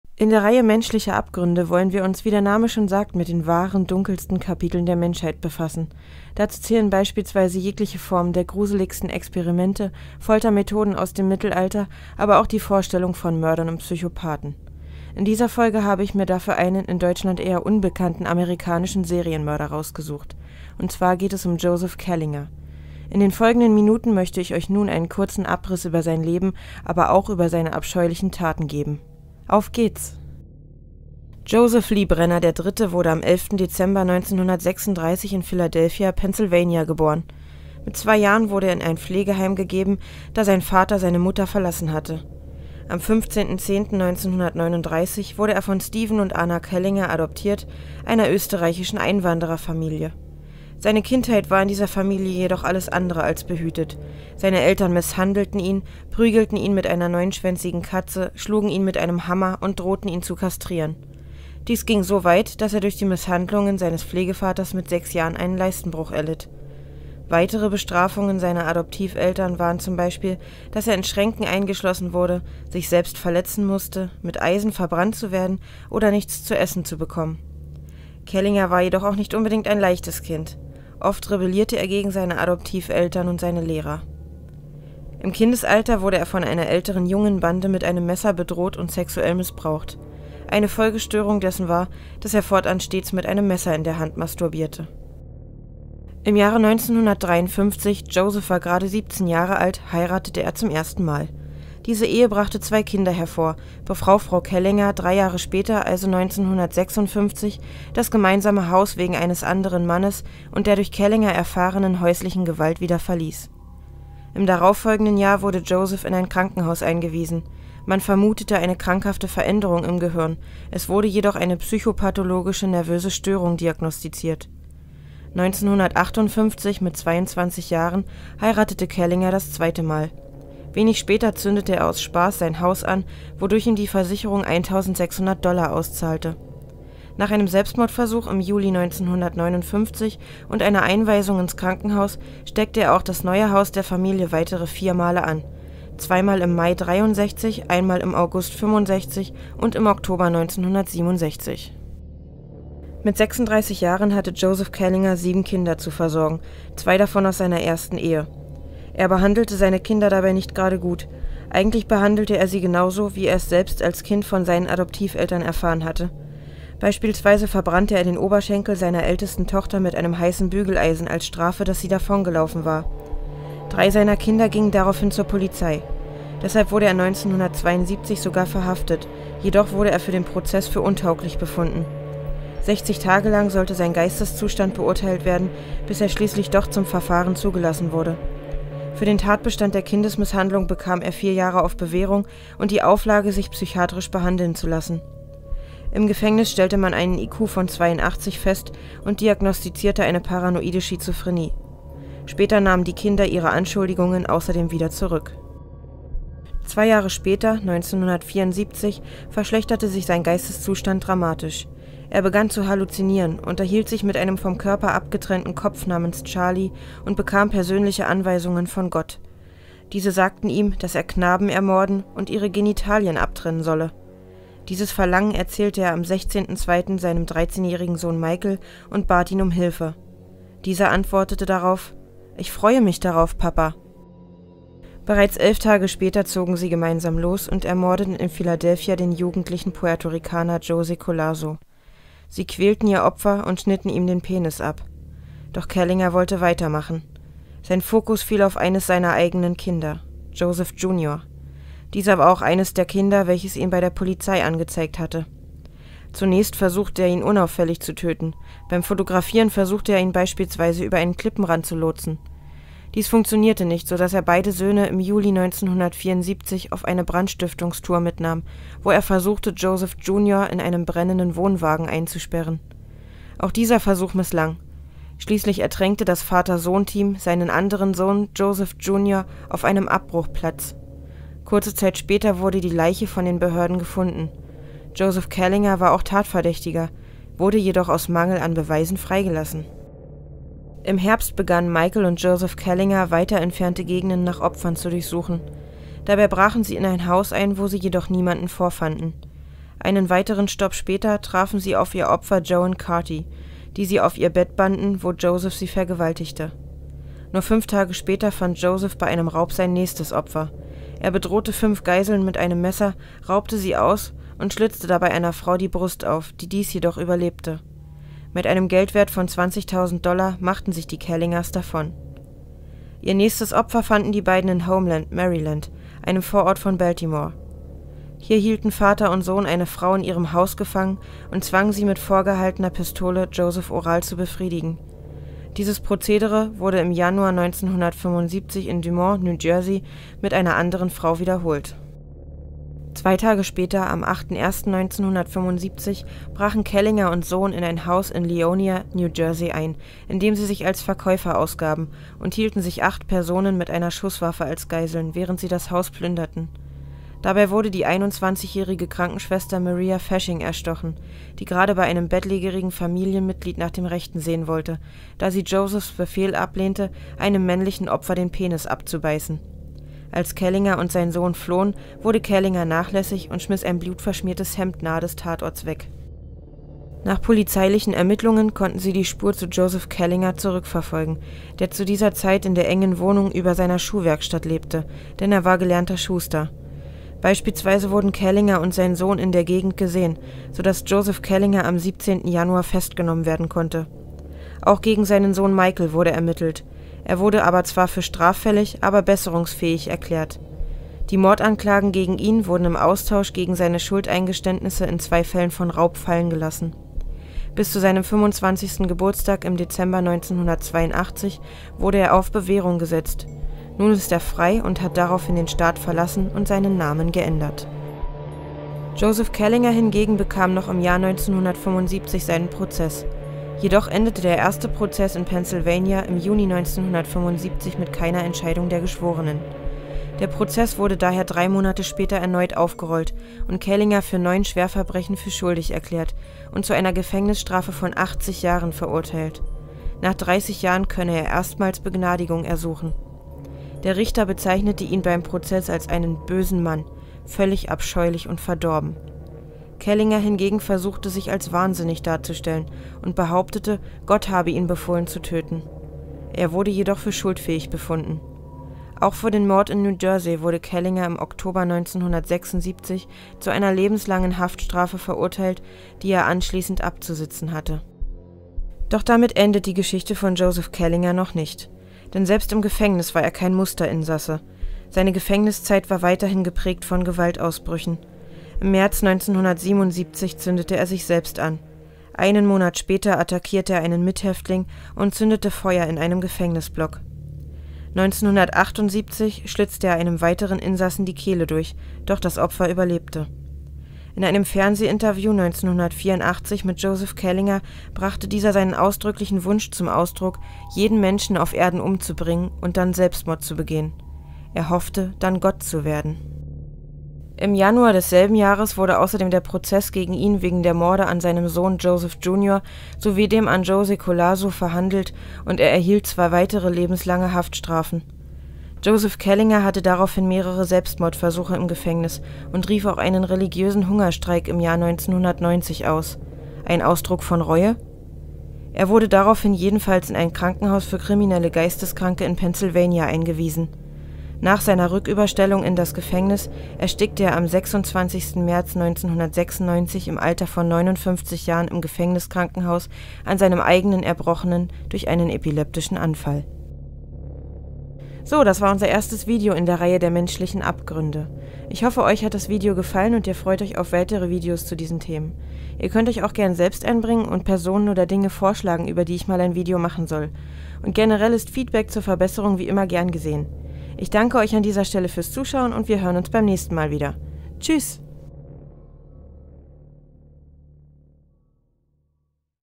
Menschliche Abgründe: Der Serienmörder Joseph Kallinger
In der Reihe "Menschliche Abgründe" wollen wir uns mit der wahren, düsteren Seite der Menschheit befassen. Hier soll es um erschreckende Experimente, schaurige Foltermethoden und um Psychopathen und Serienmörder gehen. In der heutigen Folge: Der amerikanische Serienmörder Joseph Kallinger, der gemeinsam mit seinem Sohn Michael drei Menschen umbrachte, darunter seinen eigenen Sohn Joseph jr. Und auch sonst hat dieser Serienmörder eine... nun ja, sagen wir "interessante Persönlichkeit".

Quellen:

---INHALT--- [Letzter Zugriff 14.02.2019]
„Die große Enzyklopädie der Serienmörder“ von Michael Newton, 5. Auflage, Leopold-Stocker Verlag, Graz, 2009

Thanks to the Creators!